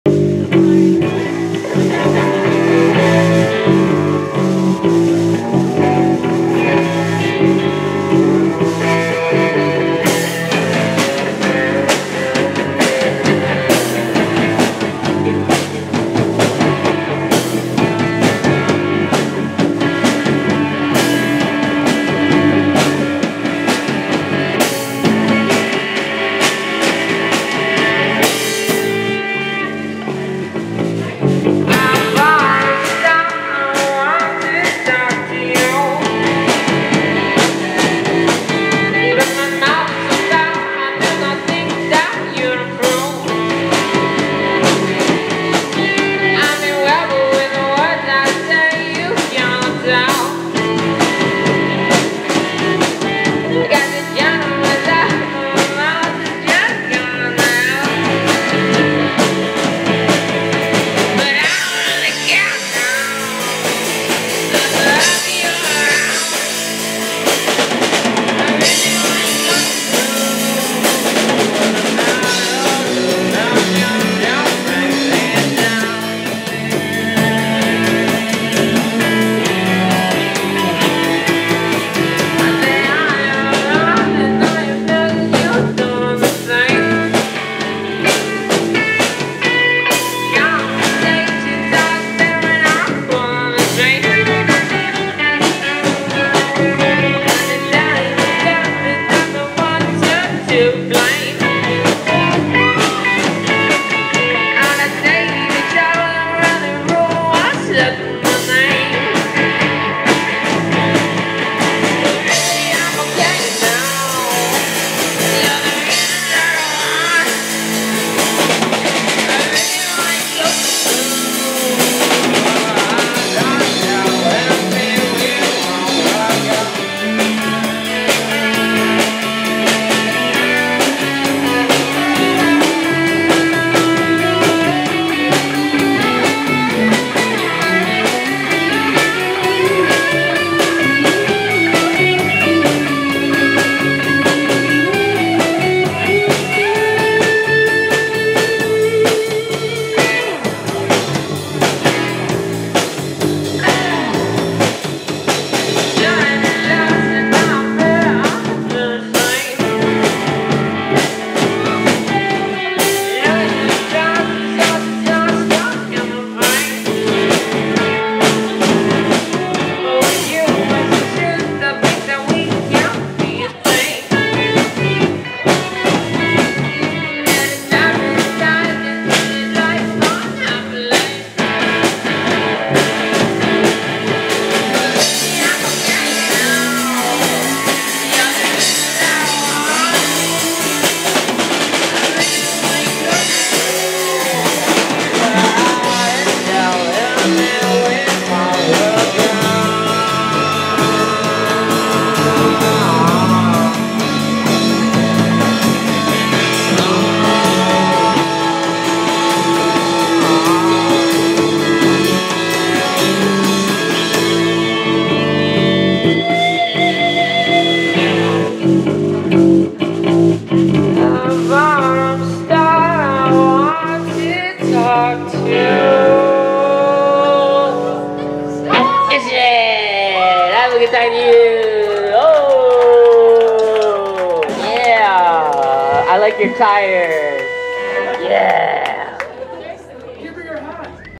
The Bougies LIVE @ The Studio at Hawaiian Brian's Nov 18, 2016
Special Thanks

The Studio @ Hawaiian Brian's
1680 Kapiolani Blvd.

Sound & Lighting Lock Lynch

The Bougies

Members

Jordan Bongolan
Kelly Bongolan
Joy Furushima
Josh Gonzalez
Evan Suhayda